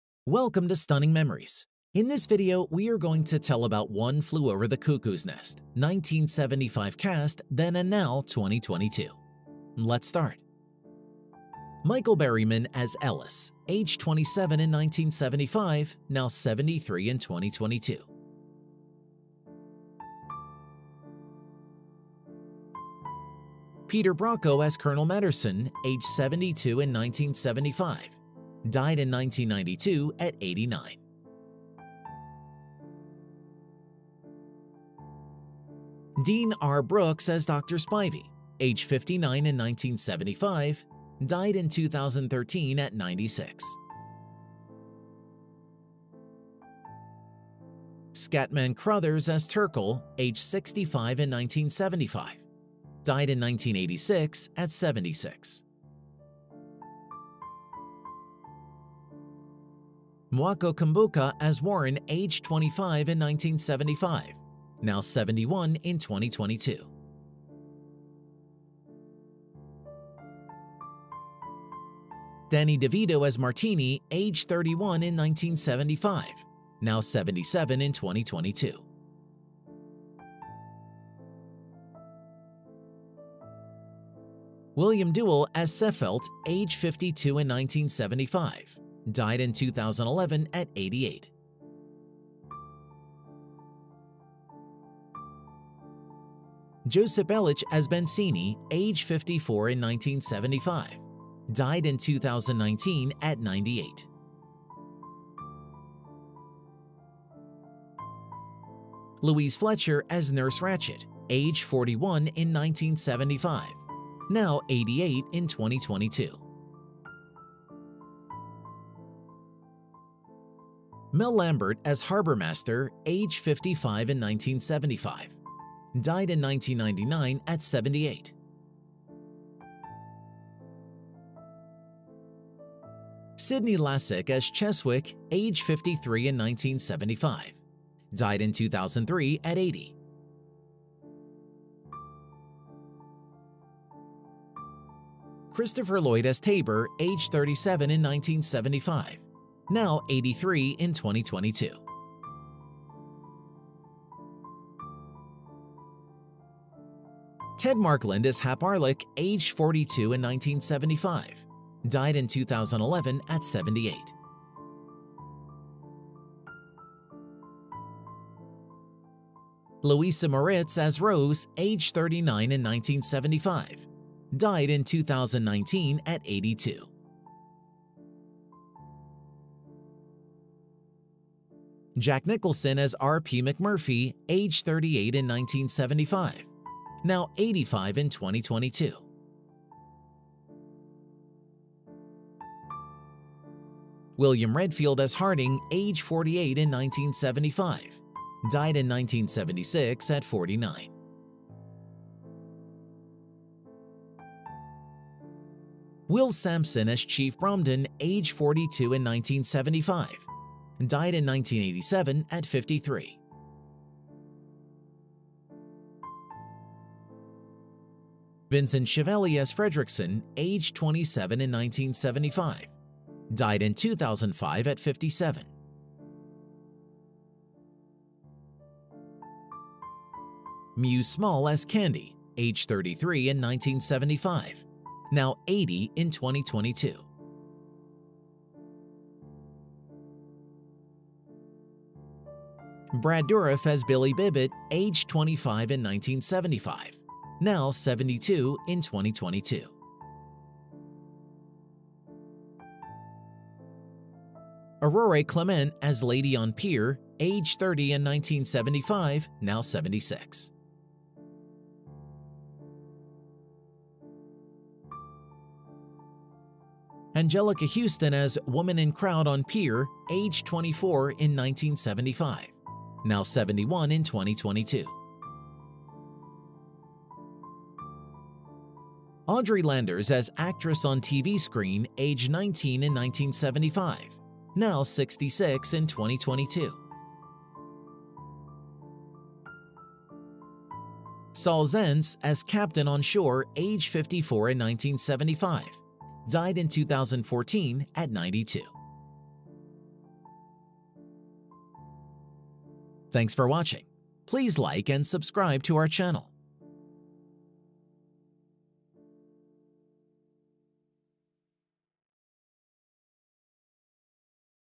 One Flew Over the Cuckoo's Nest 1975 cast then and Now 2022
One Flew Over the Cuckoo's Nest Details
1. Michael Berryman as Ellis age 27 in 1975 now 73 in 2022
2. Peter Brocco as Col. Matterson age 72 in 1975, died in 1992 at 89
3. Dean R. Brooks as Dr. Spivey age 59 in 1975, died in 2013 at 96
4. Scatman Crothers as Turkle age 65 in 1975, died in 1986 at 76
5. Mwako Cumbuka as Warren age 25 in 1975 now 71 in 2022
6. Danny DeVito as Martini age 31 in 1975 now 77 in 2022
7. William Duell as Sefelt age 52 in 1975, died in 2011 at 88
8. Josip Elic as Bancini age 54 in 1975, died in 2019 at 98
9. Louise Fletcher as Nurse Ratched age 41 in 1975 now 88 in 2022
10. Mel Lambert as Harbor Master age 55 in 1975, died in 1999 at 78
11.  Sydney Lassick as Cheswick age 53 in 1975, died in 2003 at 80
12. Christopher Lloyd as Taber age 37 in 1975 now 83 in 2022
13.  Ted Markland as Hap Arlich age 42 in 1975, died in 2011 at 78
14.  Louisa Moritz as Rose age 39 in 1975, died in 2019 at 82
15.  Jack Nicholson as R.P. McMurphy age 38 in 1975 now 85 in 2022
16.  William Redfield as Harding age 48 in 1975, died in 1976 at 49
17.  Will Sampson as Chief Bromden age 42 in 1975, died in 1987 at 53
18.  Vincent Schiavelli as Fredrickson age 27 in 1975, died in 2005 at 57
19.  Mews Small as Candy age 33 in 1975 now 80 in 2022
20.  Brad Dourif as Billy Bibbit age 25 in 1975 now 72 in 2022
21.  Aurore Clément as Lady on Pier age 30 in 1975 now 76
22.  Anjelica Huston age 24 in 1975 now 71 in 2022
23. Audrey Landers as Actress on TV Screen age 19 in 1975 now 66 in 2022
24.  Saul Zaentz as Captain on Shore age 54 in 1975, died in 2014 at 92